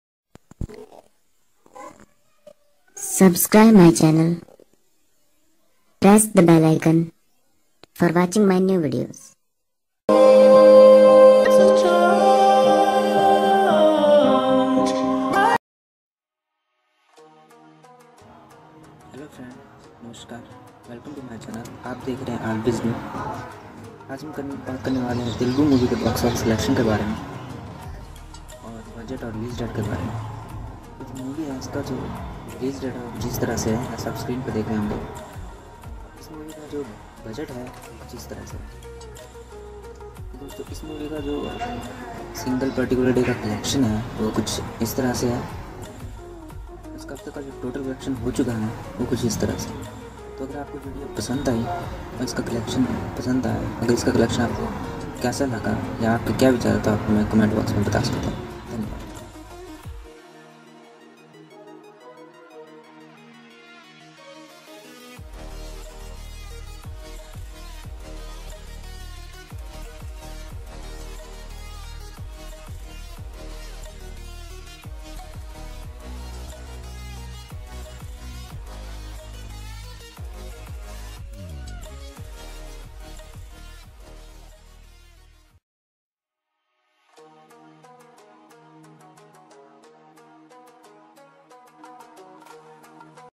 BOX office collection of Aravindha Sametha day 11/USA blockbuster/JrNTR/Till 11pm
this video is based on BOX office collection of Aravindha Sametha day 11/USA blockbuster/JrNTR/Till 11pm